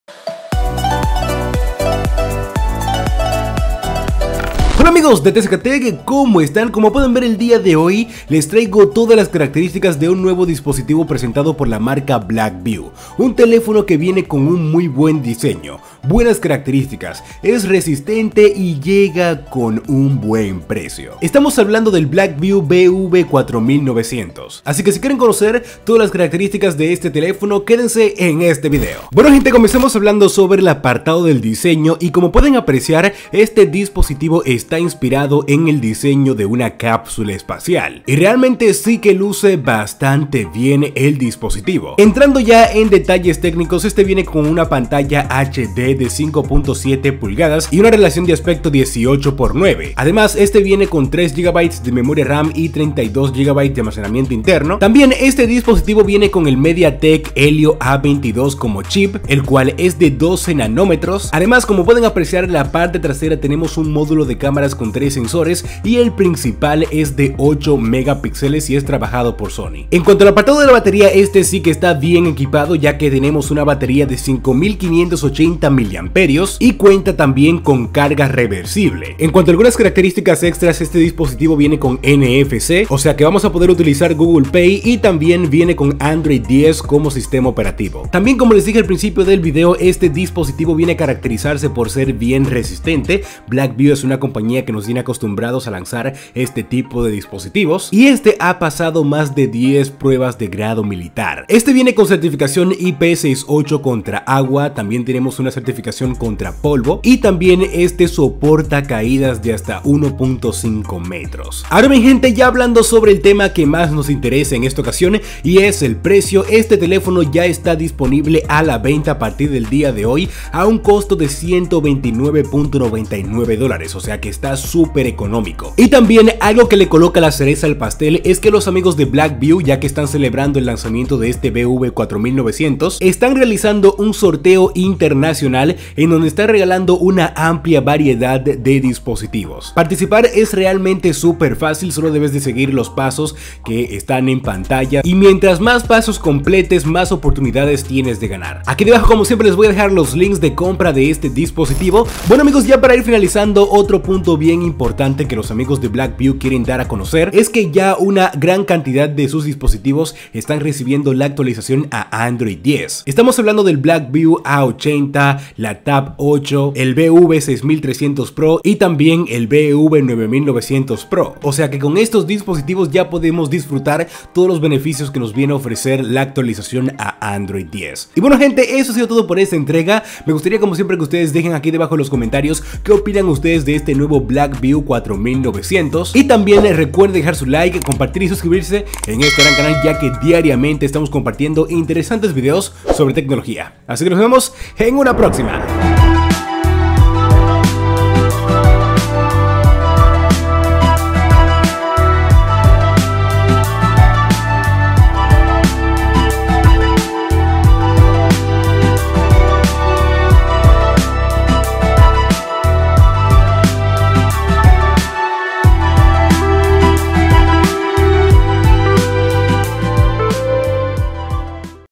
Blackview BV4900 ¡CELULAR SUPER RESISTENTE Y A BUEN PRECIO!
Blackview ha lanzado un nuevo smartphone súper económico y resistente, hablamos del BV4900, un teléfono todo terreno con Android 10 y un costo de solo $129.99 dolares.